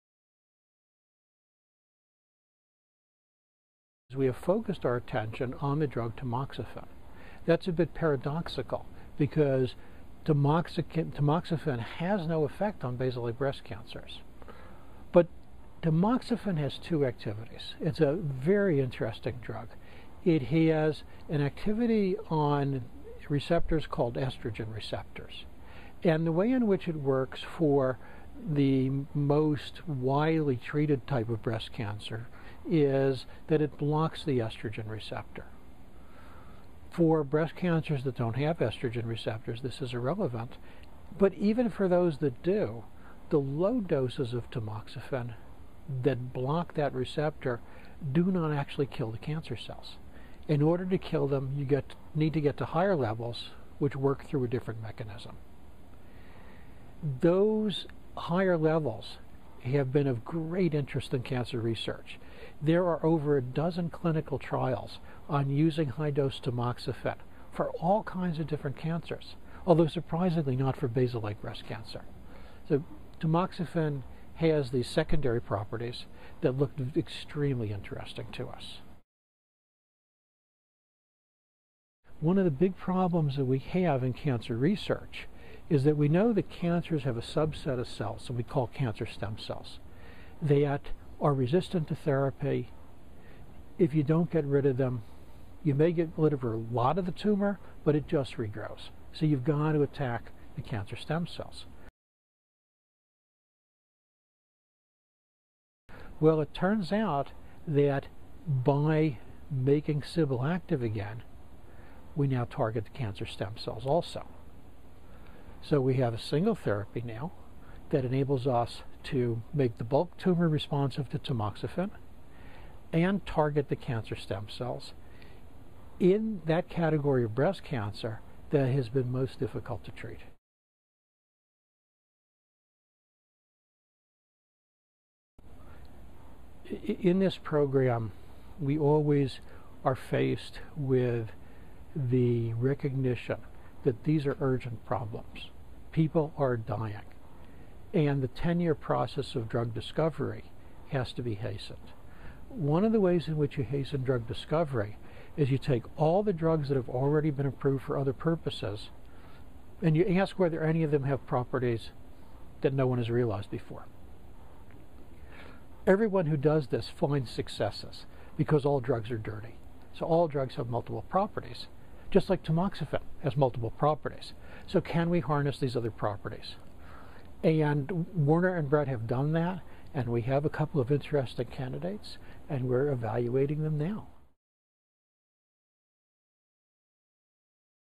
Tamoxifen's role in triple-negative breast cancer
Mark Noble, Ph.D., professor of Biomedical Genetics at the University of Rochester Medical Center, discusses the potential of tamoxifen in fighting triple-negative breast cancer (also known as basal-like breast cancer).